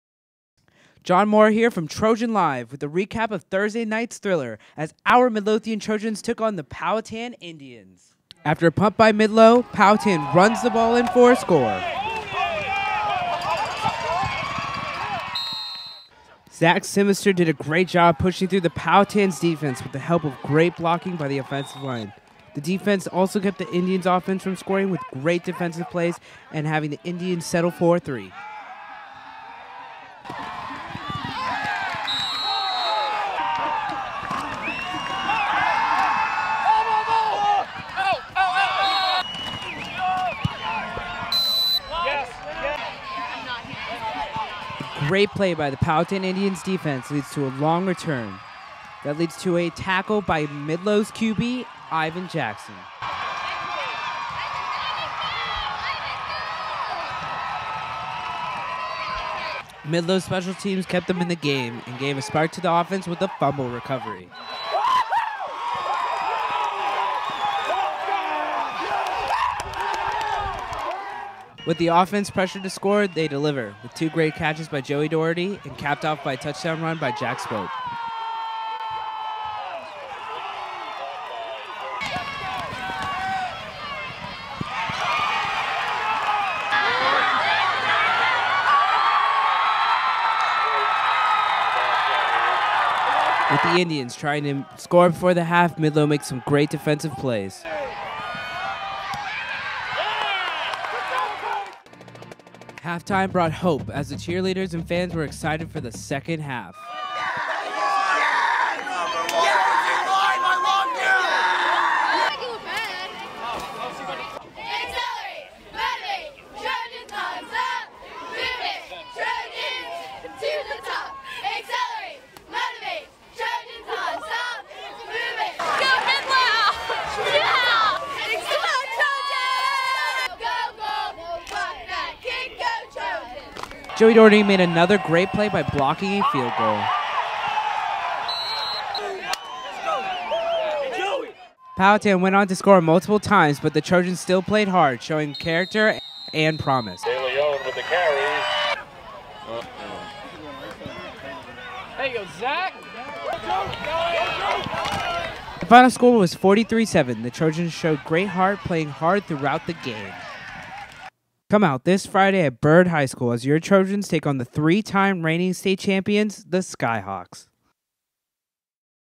Friday Night Recap: Midlothian vs. Powhatan
TrojanLive brings you a recap from Thursday night's battle between the Powhatan Indians and the Midlothian Trojans! And as always, Keep it on the "TL"